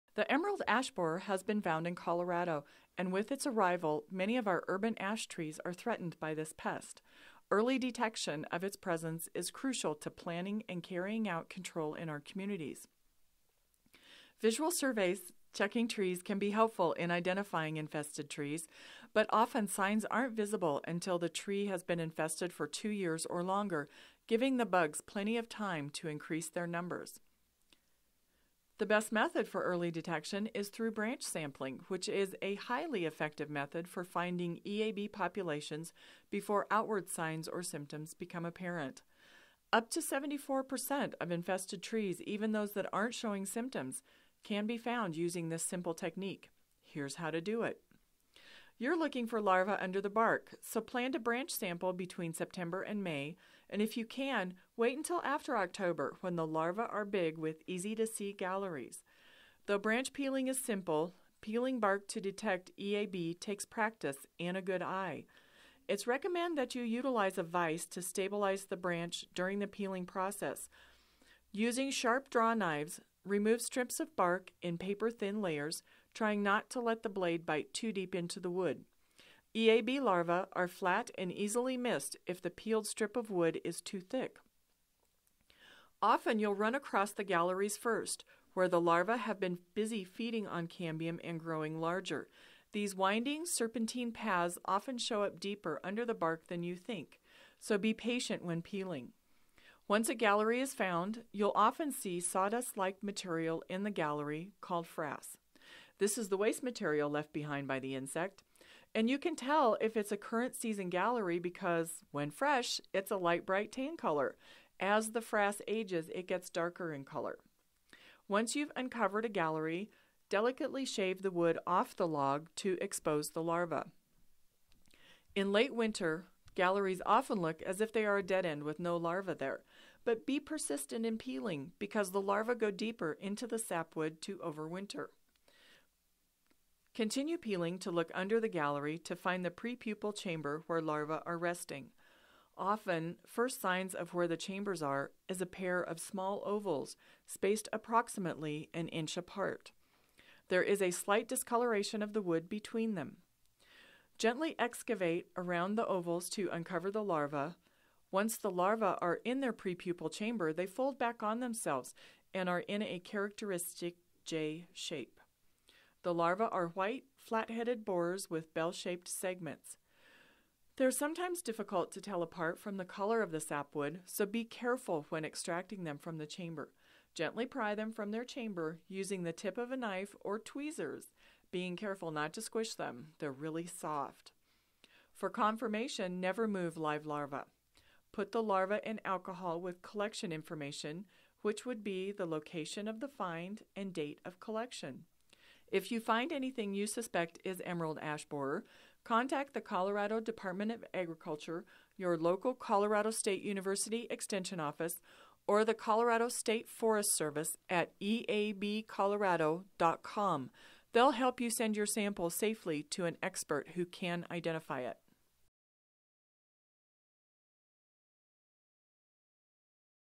EAB Bark Peeling
Introduction to Emerald Ash Borer in Colorado and how to peel bark from branches to look for EAB larvae.  From Colorado State University Extension in Boulder County and CSU Plant Diagnostic Clinic.